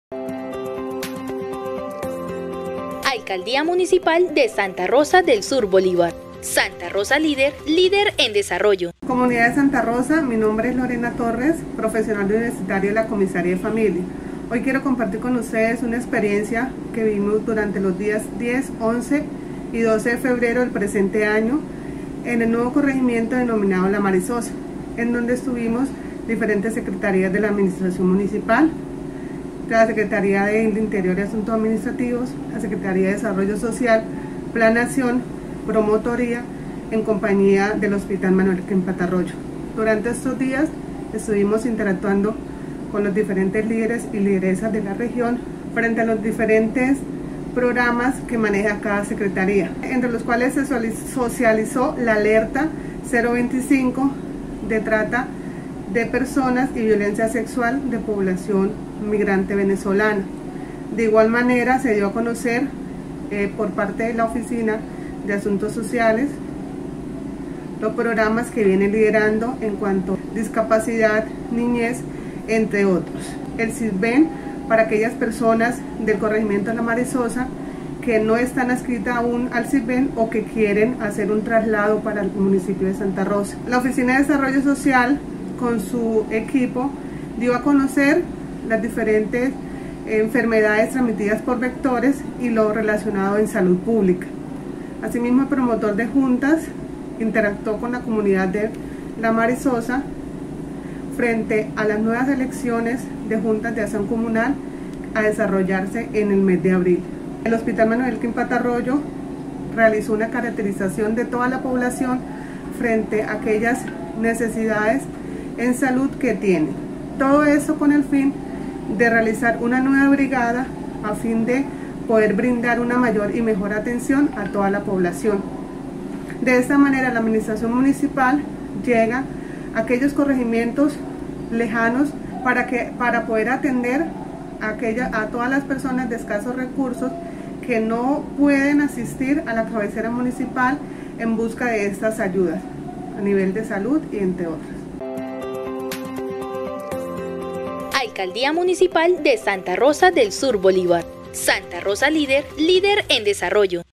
Visita al corregimiento de la Marizosa
La administración municipal Santa Rosa Líder, Líder en Desarrollo en cabeza del alcalde Fabio Orlando Mendoza Barreto y la Gestora Social Mónica Diaz Acevedo a través de nuestro equipo de gobierno en articulación con el Hospital Manuel Elkin Patarroyo hemos realizado visitas a los lugares más alejados del casco Urbano, en esta ocasión estuvimos en el corregimiento de la Marizosa, llevando nuestra oferta Institucional contando con la participación de personal de las diferentes Secretarías.